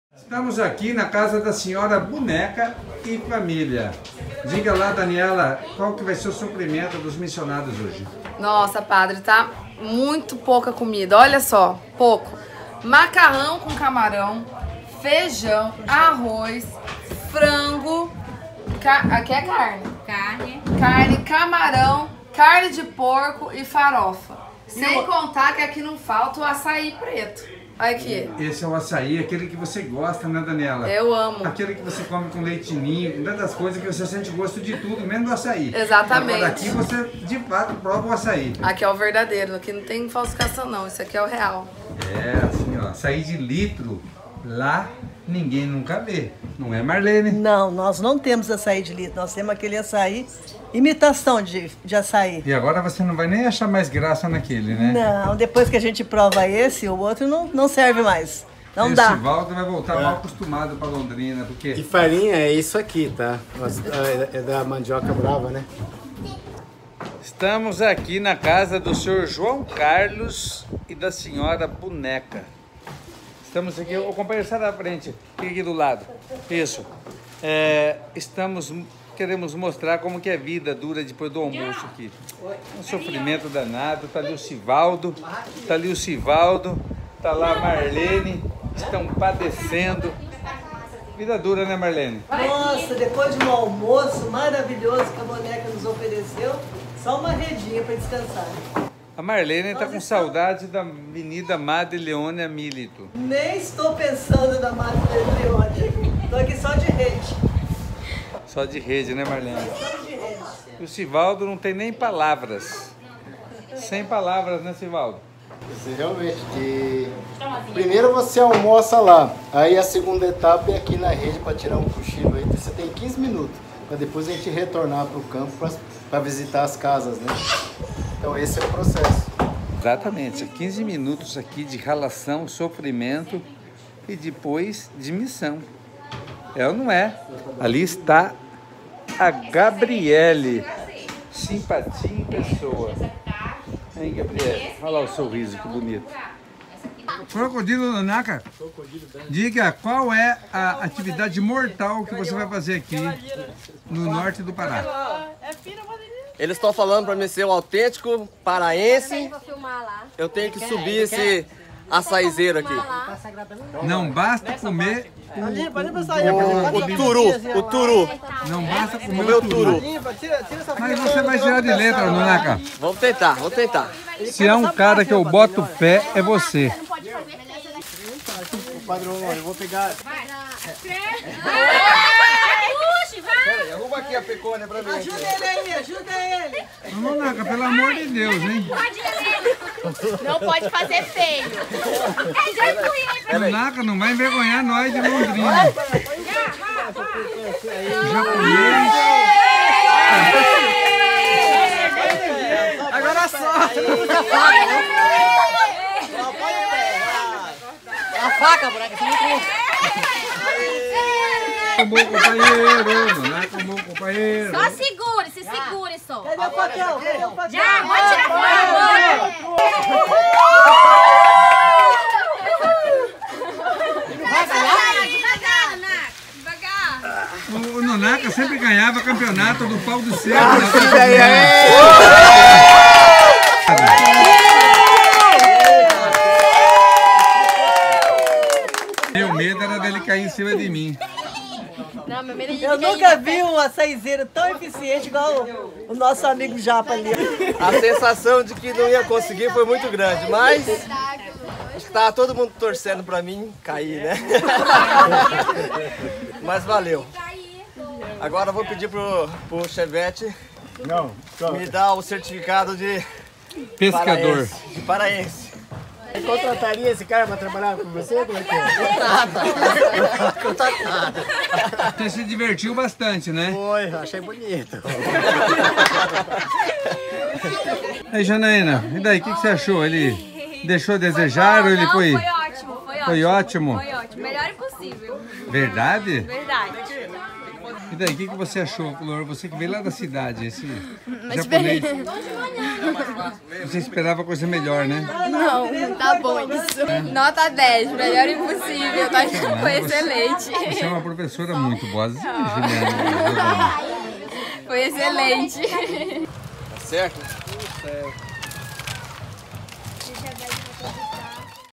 ABAETÉ 2018 ALMOÇO NA FAMÍLIA SILVA
Missionários de Londrina almoçam na residência da Família Silva, da Comunidade Nossa Senhora Aparecida das Ilhas de Abaetetuba PA. 19/07/2018.